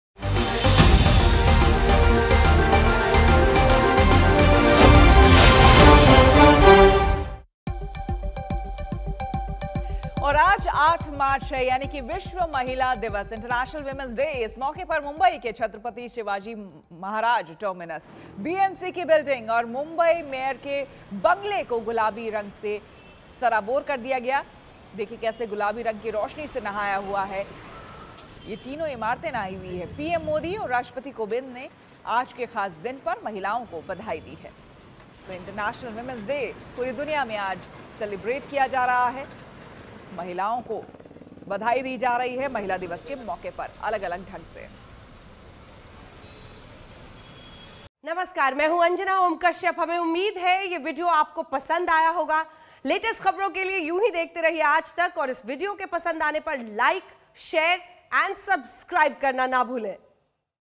International Women's Day:BMC Building, CST और Mayor House गुलाबी रोशनी में नहाएं,PM Modi ने दी बधाई
International Women's Day: आज विश्व महिला दिवस है.इस मौके पर PM Modi और President Ramnath Kovind ने बधाई दी. वहीं Maharashtra में BMC Building, CST और Mayor House को गुलाबी रोशनी से सजाया गया.